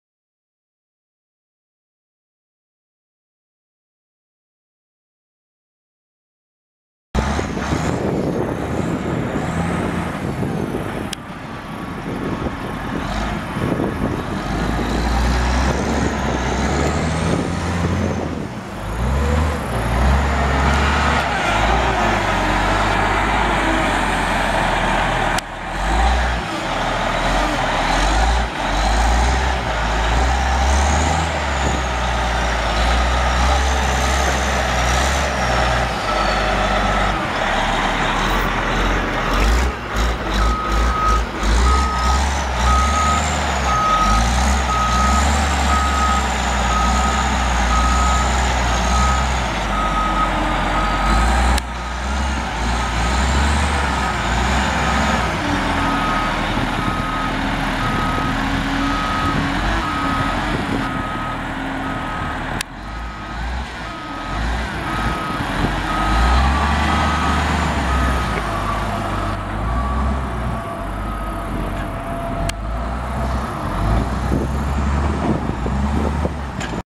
2004 Caterpillar 980G wheel loader for sale | sold at auction July 31, 2014
2004 Caterpillar 980G wheel loader
33,154 hours on meter
Caterpillar 3406 diesel engine
300 HP
Powershift transmission
4F-4R gears
EROPS
AC and heat
Tilt
Suspension seat
CB radio
11&#039;W bucket
Pin hitch
29.5R25 tires
66,014 lbs. base operating weight
Serial CAT0980GVAWH01922

It was located in Beasley, TX, and we sold it to the highest bidder for $48,950.